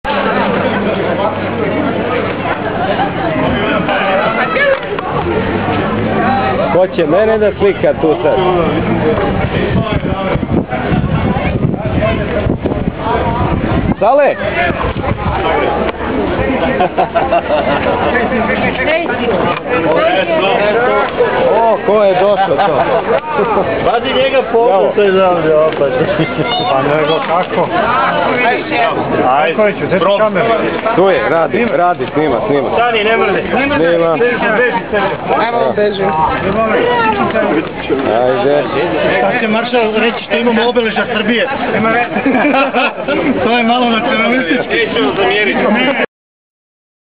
Pionirke and Tito in Vrsac Parade 20 09 2009
Parade in Vrsac for vine festival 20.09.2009. Participants were shown in action... Bicyclists liked to have photo with Tito, Jovanka and young pioneers..